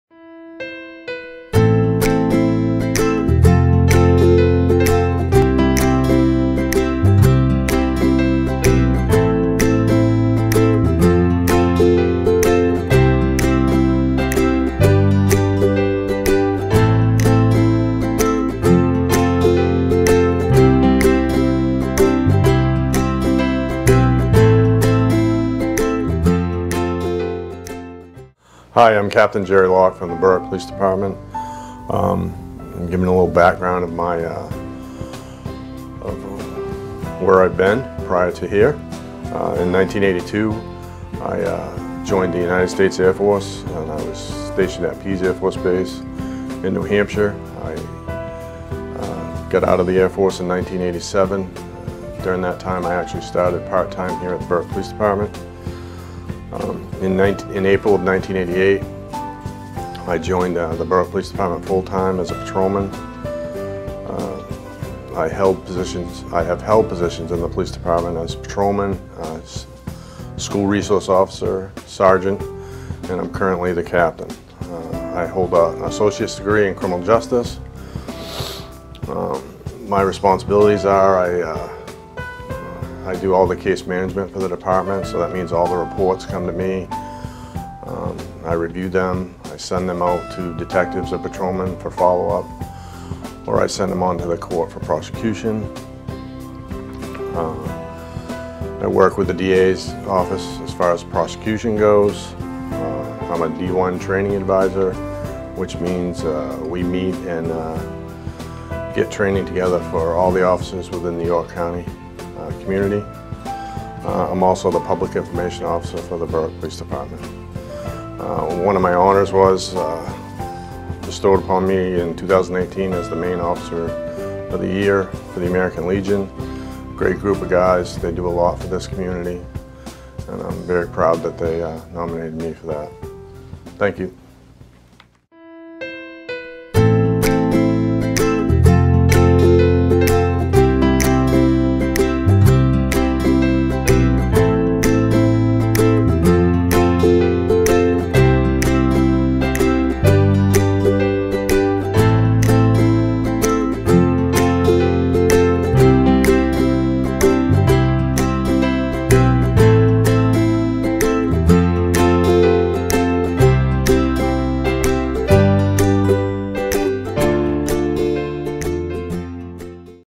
Meet Captain Jerry Locke of the Berwick PD
BCTV records Berwick Police Officers telling residents about their background, training and responsiblities in the Berwick Police Department.